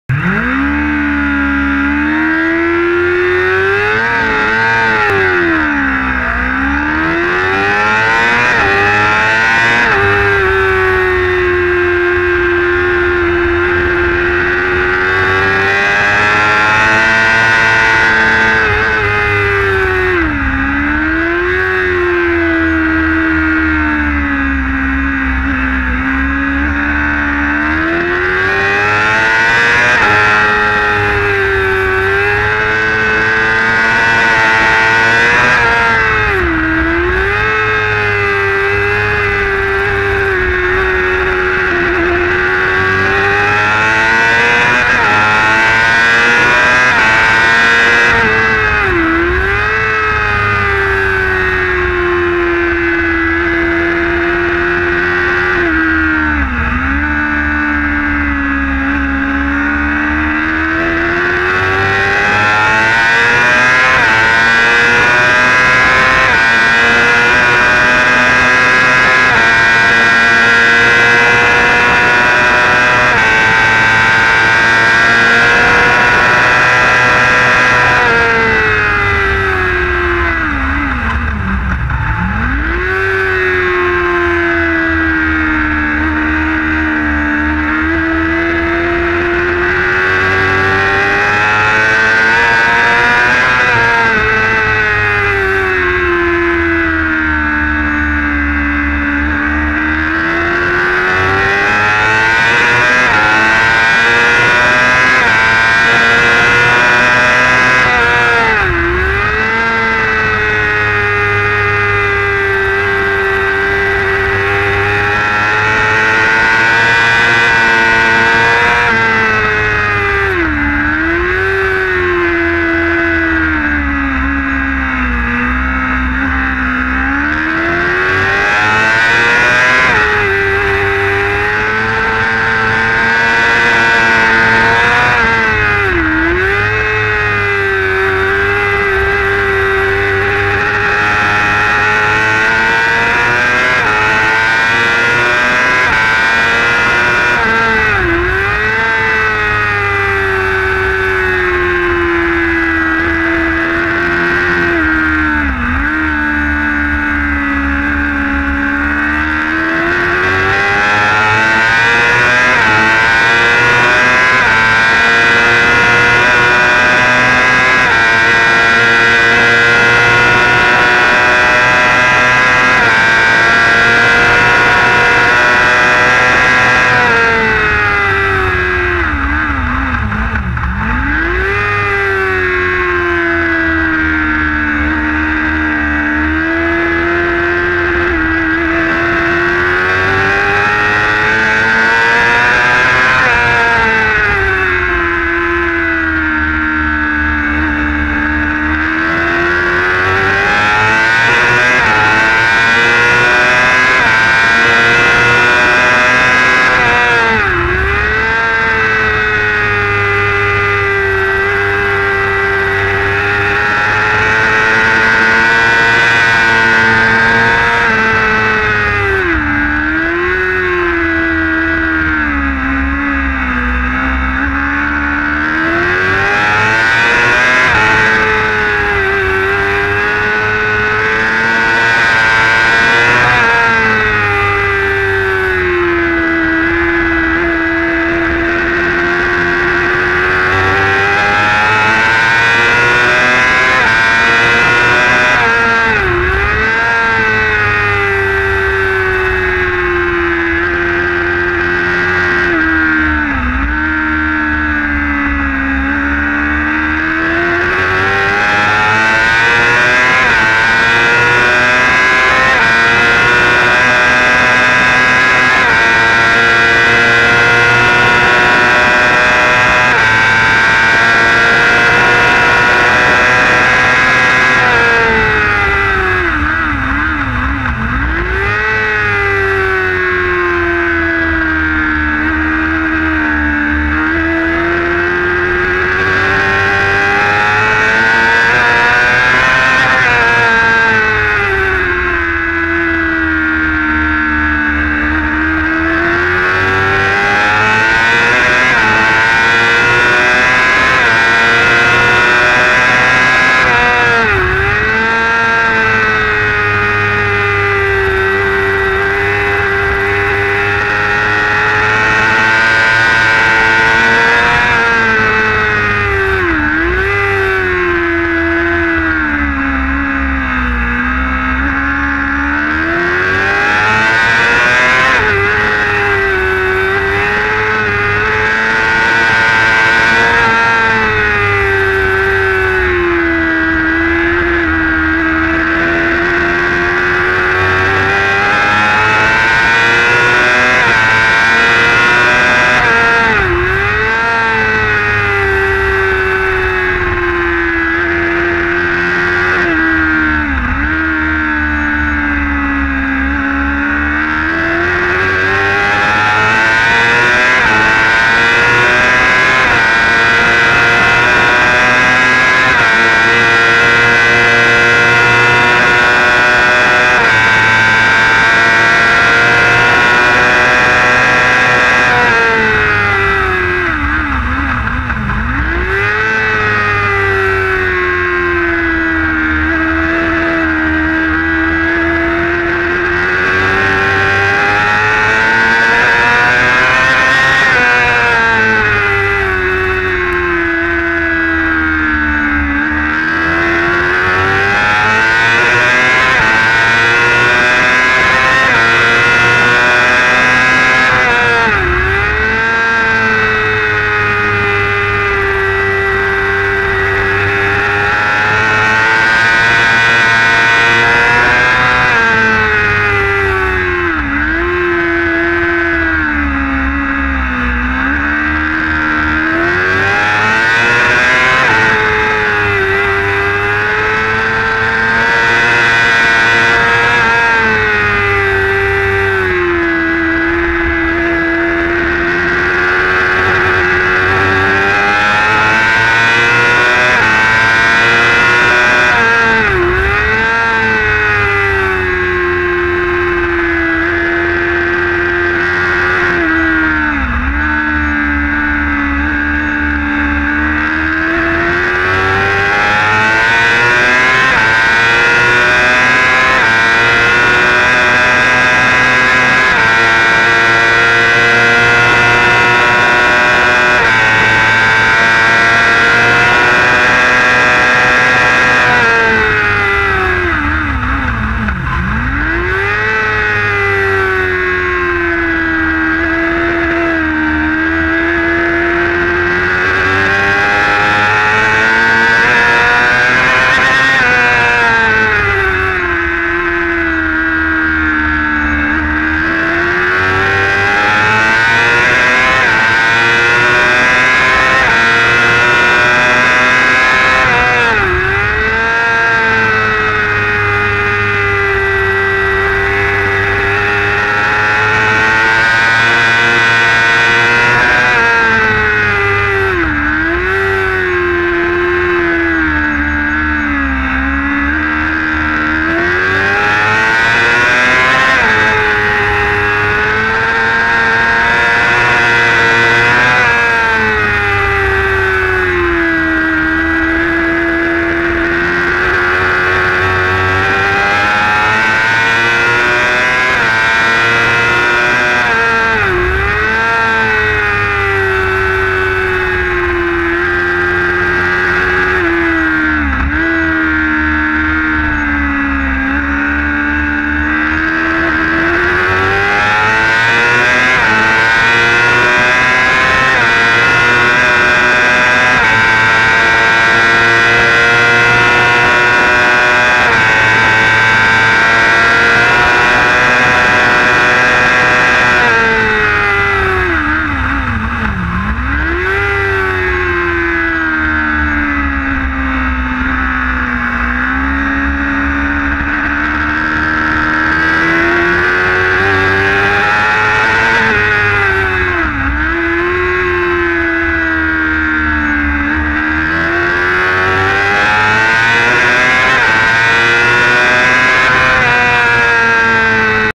C superstock WERA race @ Road Atlanta 6/8/2013
C superstock WERA race @ Road Atlanta 6/8/2013 onboard with novice #191